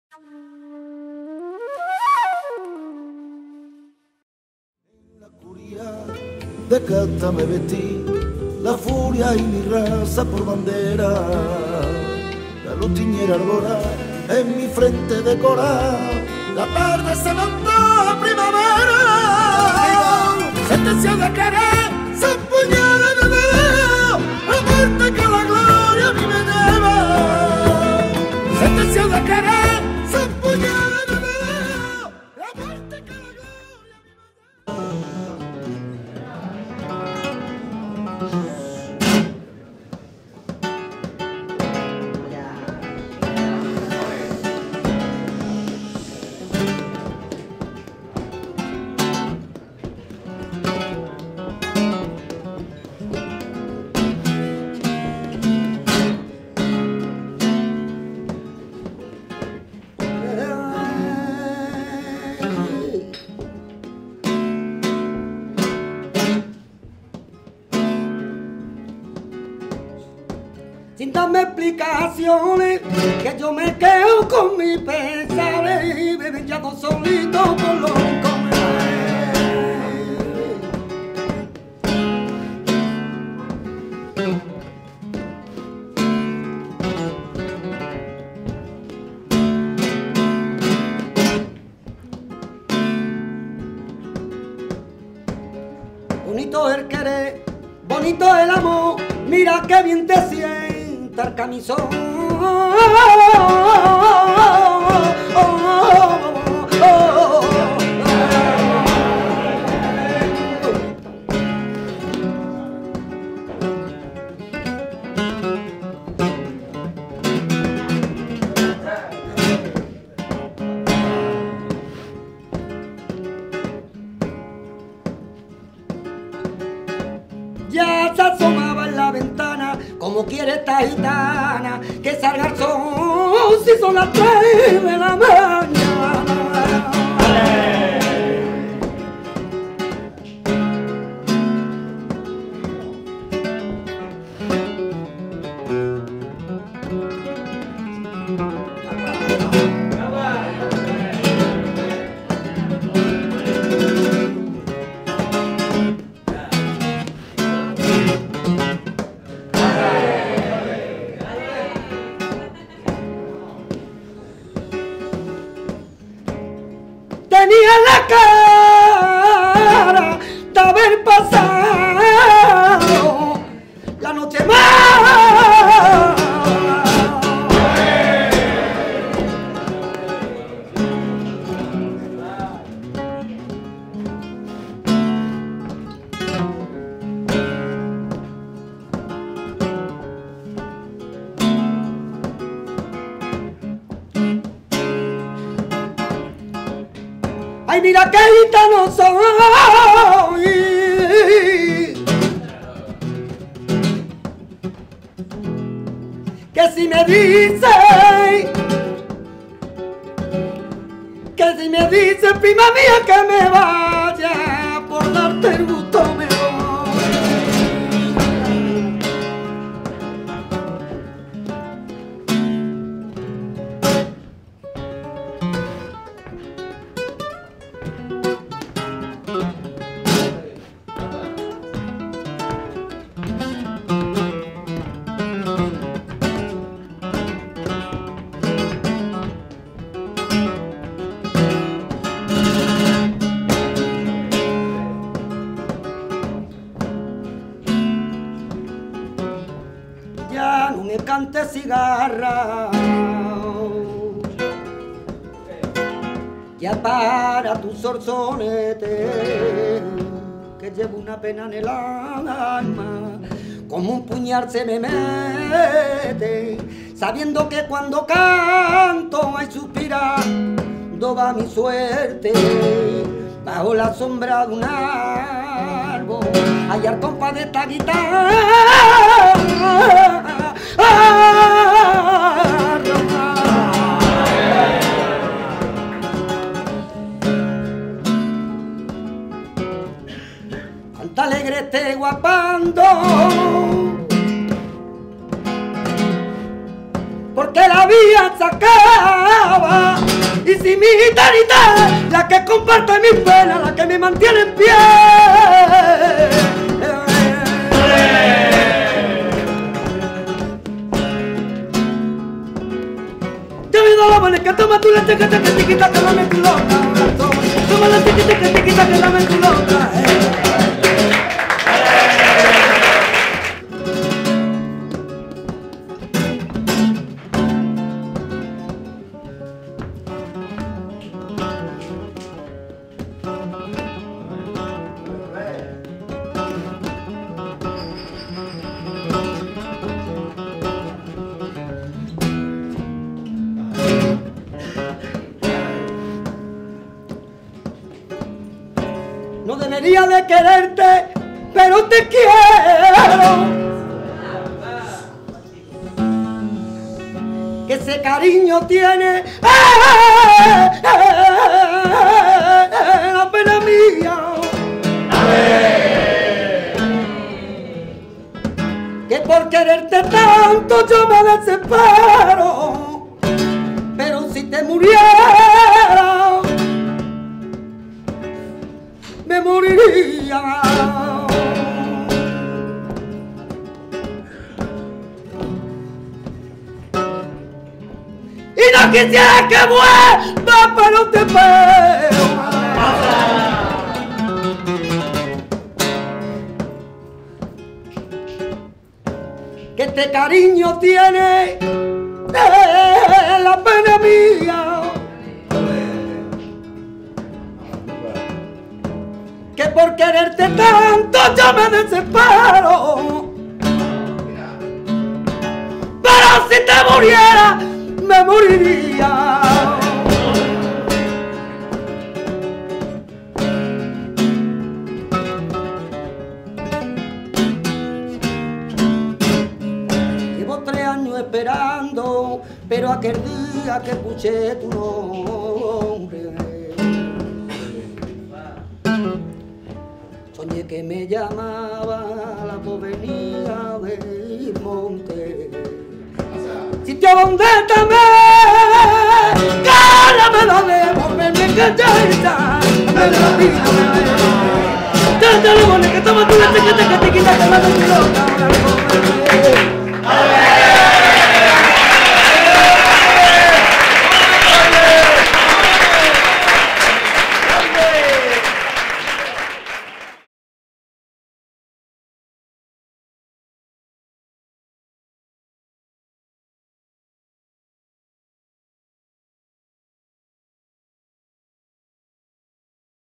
Duquende y Manuel Valencia en el Almíbar - Bulerías
El último recital de la pasada temporada. Duquende, historia viva del Flamenco... se apretó los machos en nuestra casa y nos entregó todo lo que llevaba. Gracias, Leyenda.

¡Viva el almíbar!